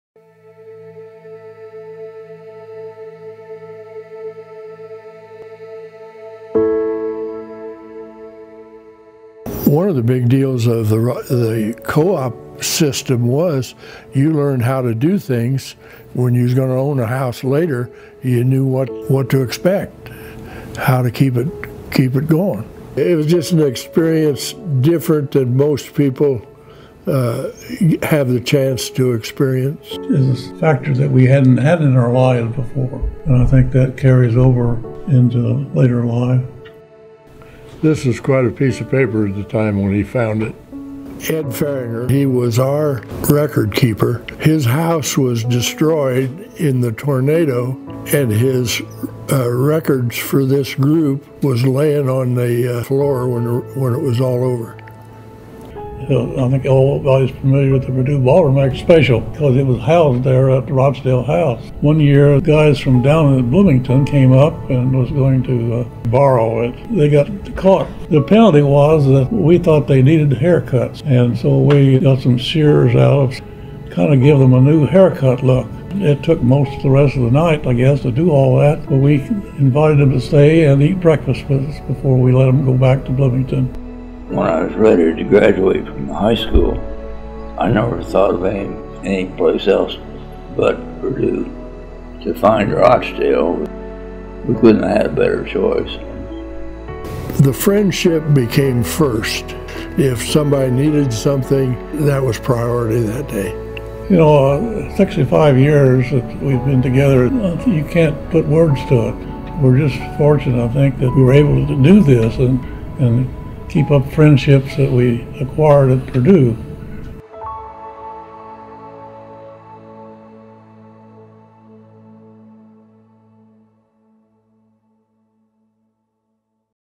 Rochdale Reunion
Take a look at this video from the 65th annual Rochdale Cooperative House picnic, and we promise you’ll laugh, cry, or maybe both!

A huge thank you goes to Tom Boese (A’49) for inviting us into his home for the event and to Phil Carrington (A’50) and Don Ashbaugh (ECE’49) for sharing their Purdue stories with us.